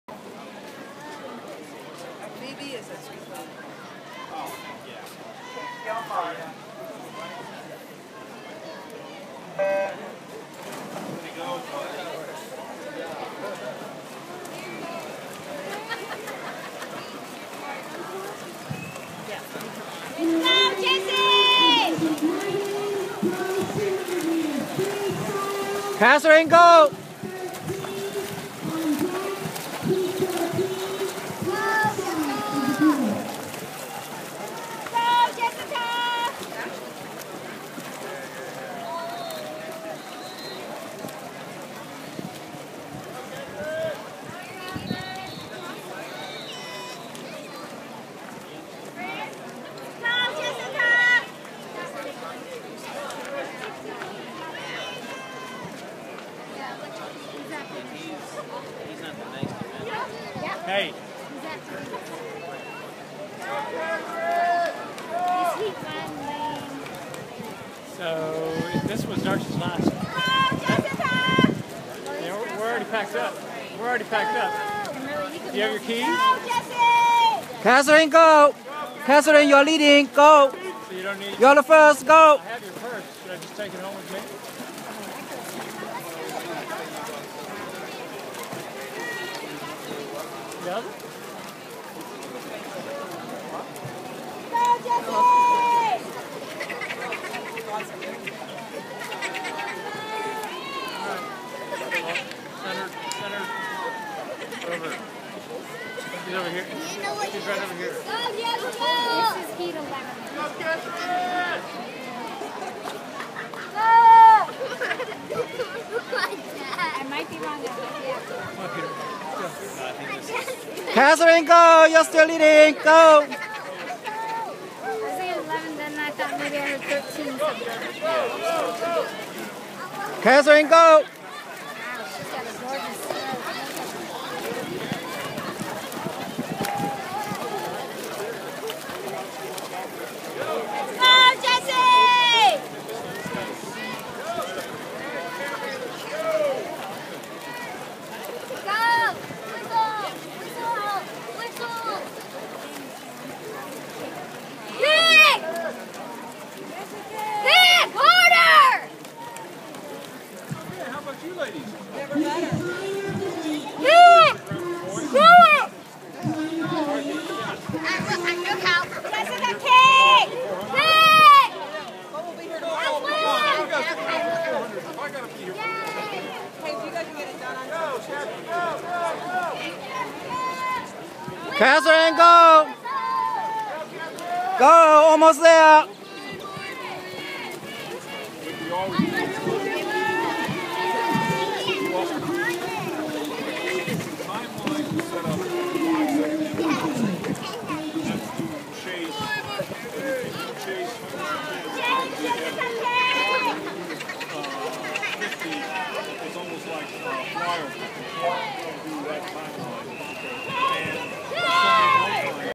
Kathryn's first 200m. Free Style swim race
May 12,2012, her 1st race in this event at First Colony Aquatic Center